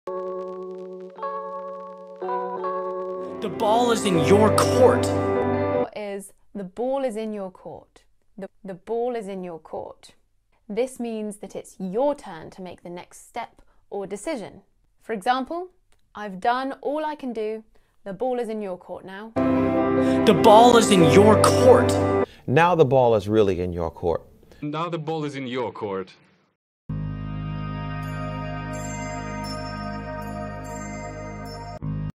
Learn English Idioms: The ball is in your court - Meaning & Pronuntiation
The ball is in your court

The idiomatic expression originated in America in the early 19th century. The concept of symbolic “ball” comes from the sports world, such as tennis or/and basketball. It shows the ball being thrown around, and the person who is occupying the court has the responsibility to throw it off to the other side. The phrase also subtly hints towards getting rid of the ball (the task or the next move) as early and as swiftly as possible in the same context.

What does it mean? It’s up to you

How do you use it? It’s your move now, but this idiom refers to life rather than a sport. If you’ve got the ‘ball,’ the decision is yours and someone is waiting for your decision.